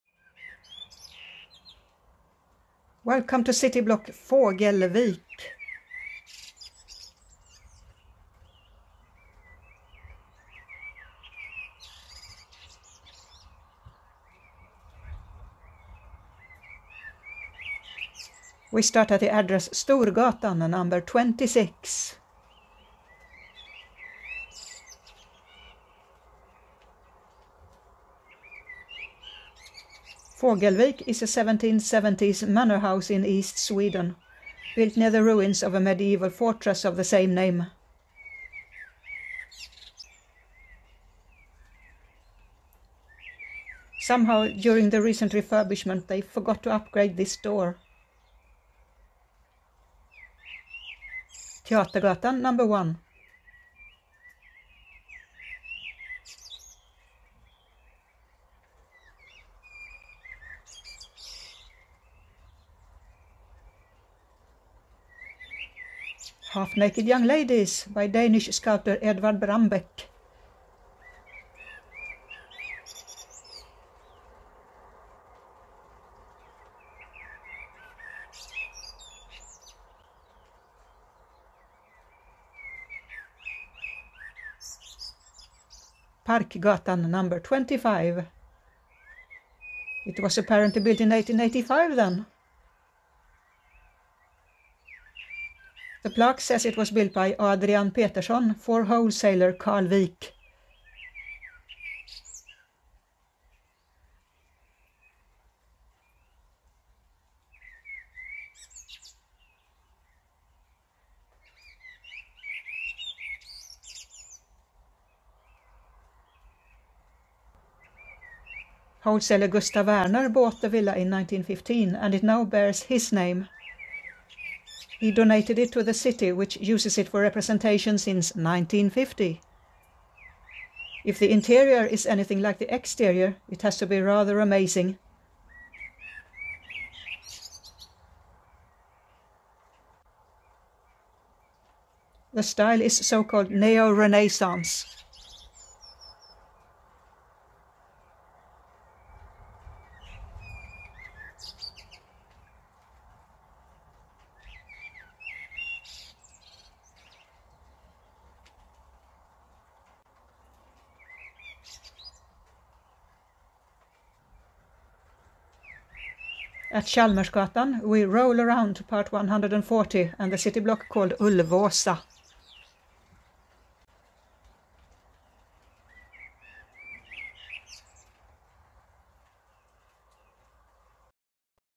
Fasadprojektet del 139: kvarteret Fågelvik
Documenting architectural decorations in Gothenburg, an ongoing project. This episode is from Vasastaden (formerly Lorensberg).
Narration:
Welcome to city block Fågelvik.
We start at the address Storgatan 26.
Fågelvik is a 1770s manor house in east Sweden, built near the ruins of a medieval fortress of the same name.
Somehow, during the recent refurbishment they forgot to upgrade this door.
Teatergatan 1.
Half-naked young ladies by Danish sculptor Edvard Brambeck.
Parkgatan 25.
It was apparently built in 1885, then.
The plaque says it was built by Adrian Peterson for wholesaler Carl Wijk.
Wholesaler Gustaf Werner bought the villa in 1915 and it now bears his name.
He donated it to the city which uses it for representation since 1950.
If the interior is anything like the exterior it has to be rather amazing.
The style is so-called neo-renaissance.
At Chalmersgatan, we roll around to part 140 and the city block called Ulvåsa.